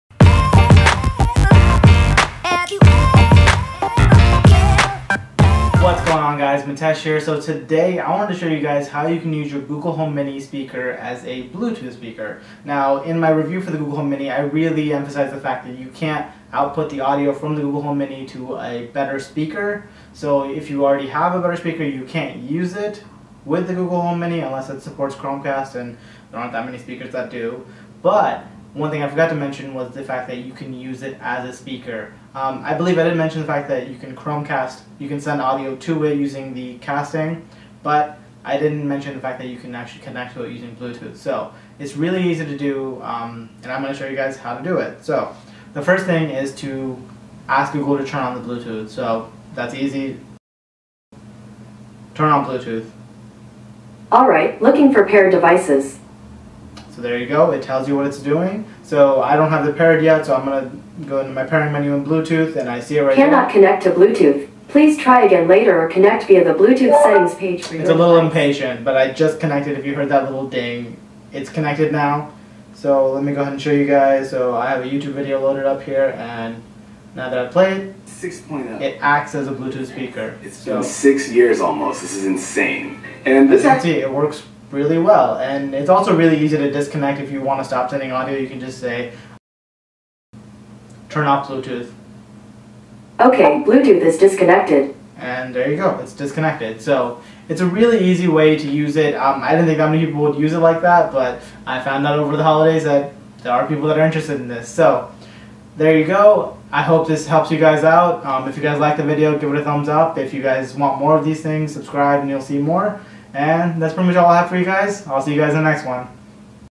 Google Home as a Bluetooth Speaker
Second Video in my Tech Tip series. In this I'll show you how to use the Google home ( or Home Mini or Home Max) as a Bluetooth speaker. It really couldn't be easier and this video will show you how to do that in just under 2 minutes.